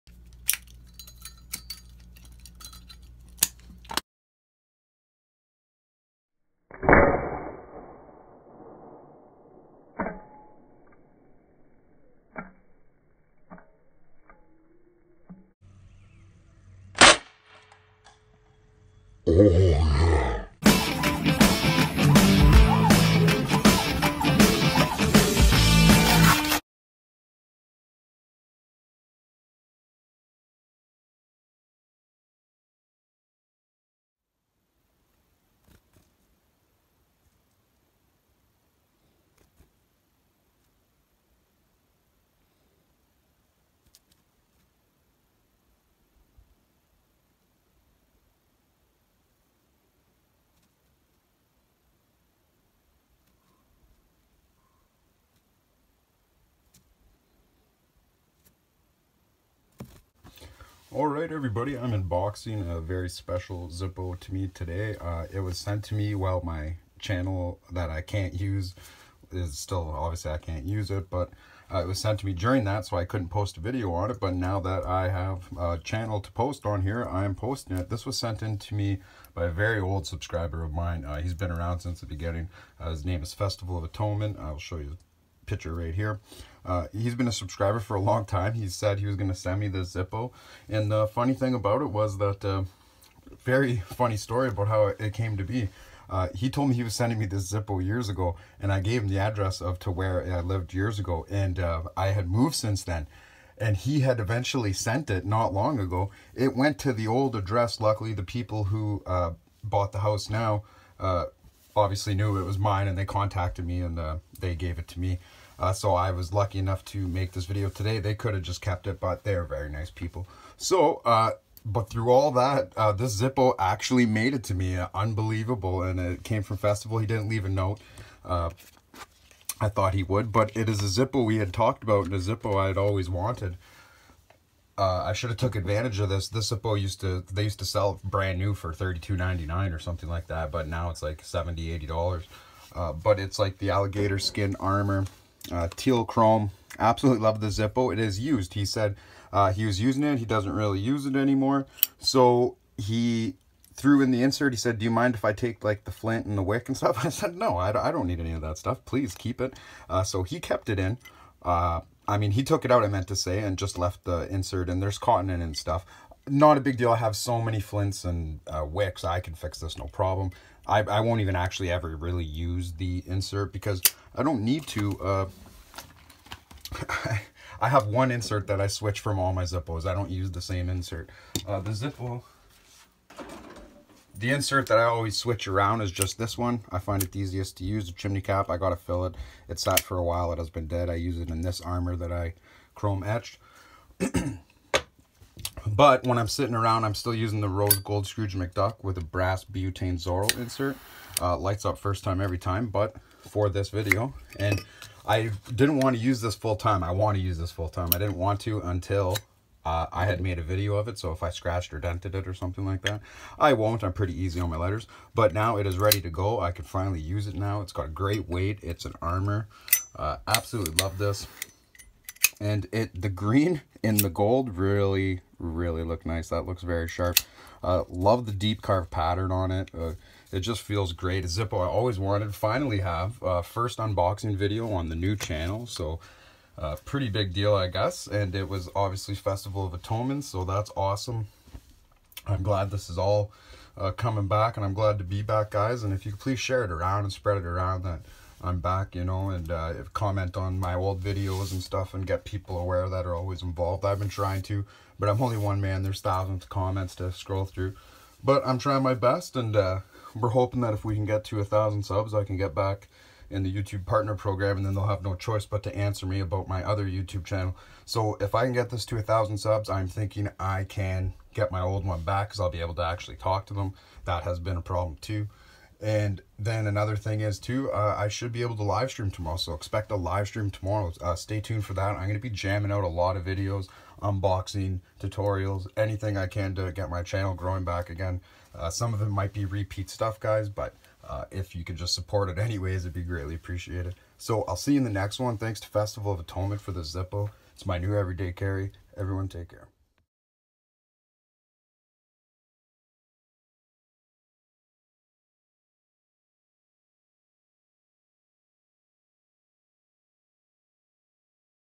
UnBoxing Armor Zippo Gator Skin Deep Carve
In this video I will be unboxing the armor zippo with a Gator skin deep carve engrave with teal chrome.